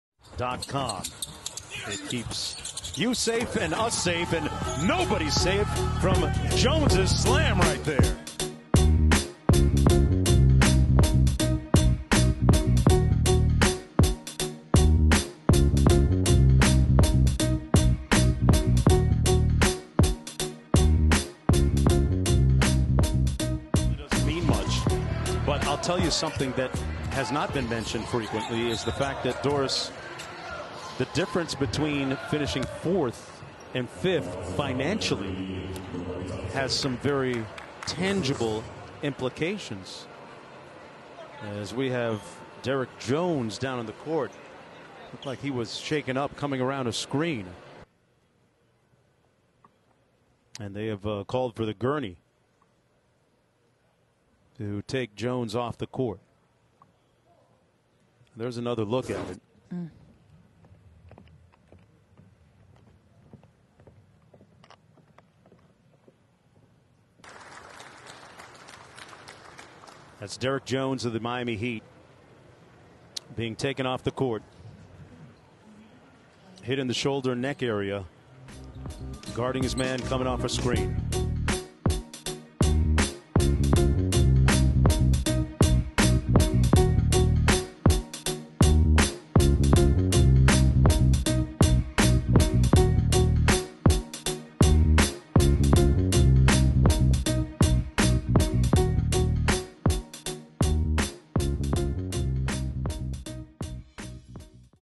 Prayers Up: Derrick Jones Jr. throws down jam vs the Pacers, but is later carted off the court
Miami Heat star Derrick Jones Jr. soars early for a thunderous jam vs the Pacers, but is later laid low by a screen and has to be carted off...